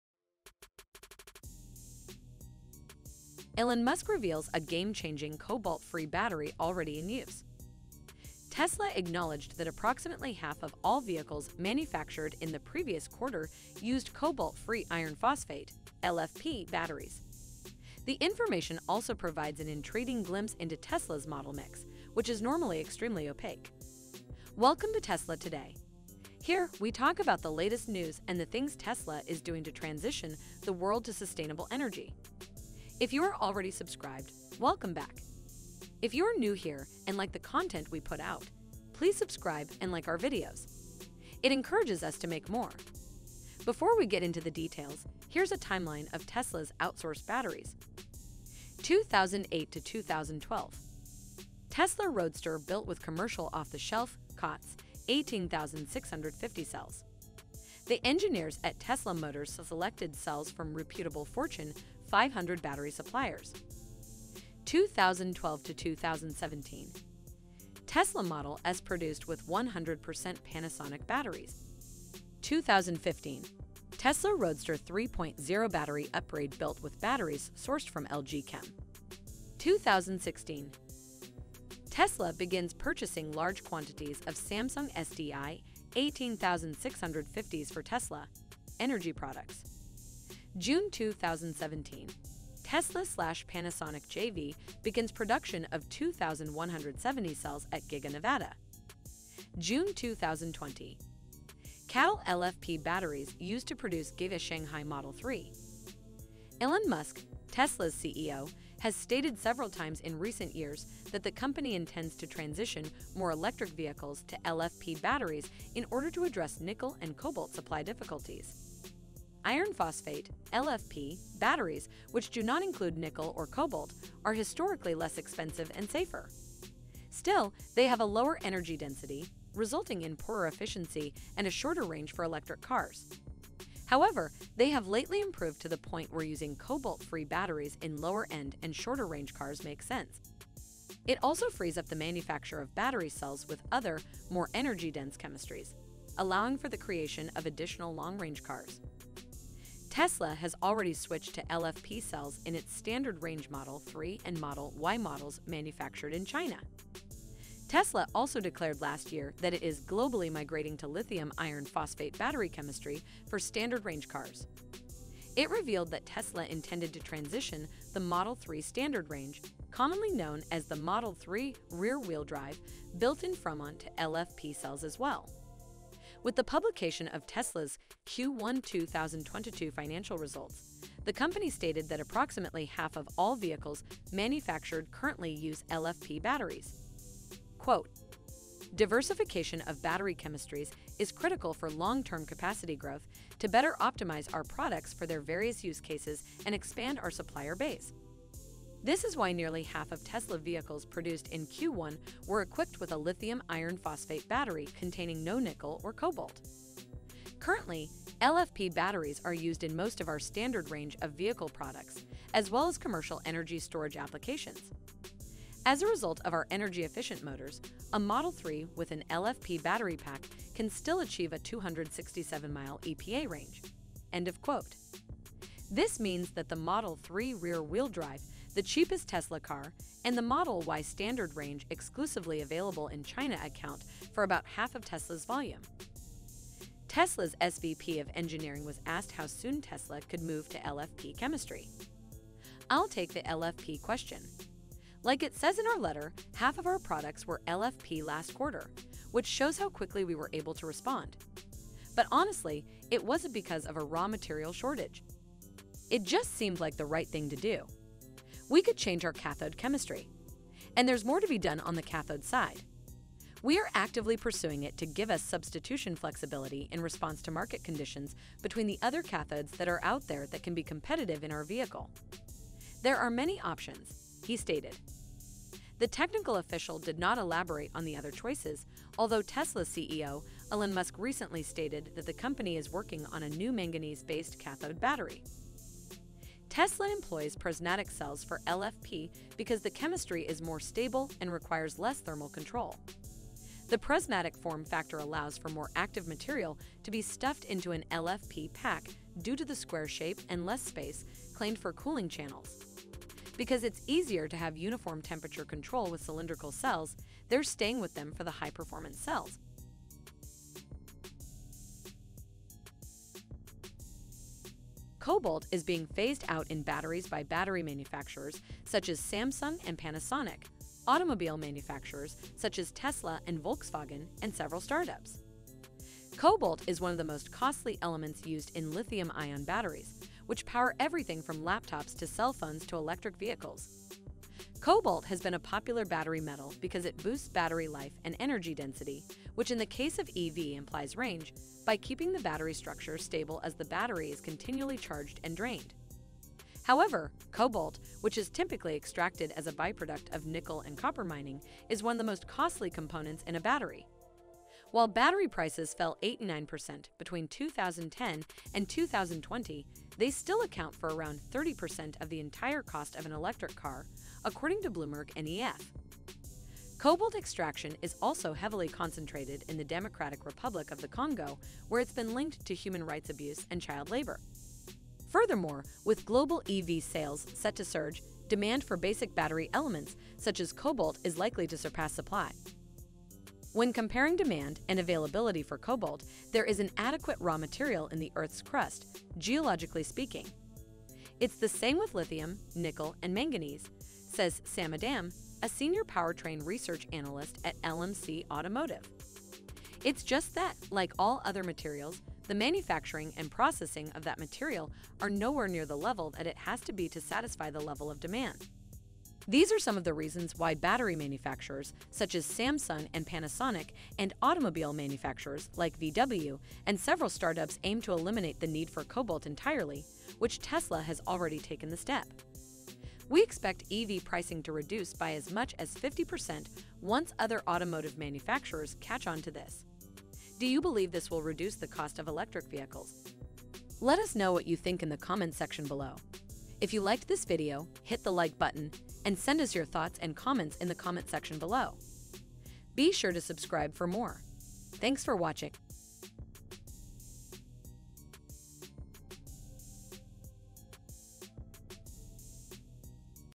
Could this New Cobalt-free battery slash EV prices by half?
Tesla acknowledged that approximately half of all vehicles manufactured in the previous quarter used cobalt-free iron-phosphate (LFP) batteries.

Cobalt, which is typically extracted as a byproduct of nickel and copper mining, is one of the most costly components in a battery. While battery prices fell 89 percent between 2010 and 2020, they still account for around 30 percent of the entire cost of an electric car,

Today, we take a look at how some new battery breakthroughs want to make EVs cheaper.